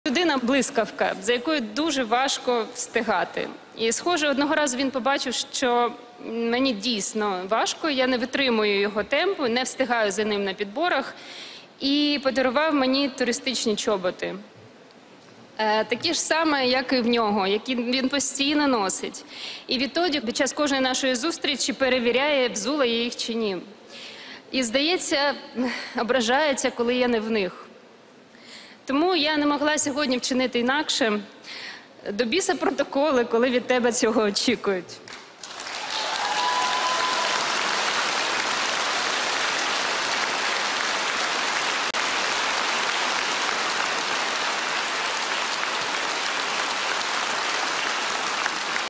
ДО БІСА ПРОТОКОЛИ, коли від тебе цього вимагають! Зеленська ВРАЗИЛА УСІХ — Я НЕ МОГЛА ВЧИНИ ІНАКШЕ
Перша леді України Олена Зеленська зізналася, що порушила протокол, з'явившись 22 серпня на сцені під час вручення премії "Національна легенда України" у брючному костюмі, вишиванці й туристичних черевиках. За словами Зеленської, ці черевики подарував їй американський бізнесмен, філантроп, Говард Баффетт.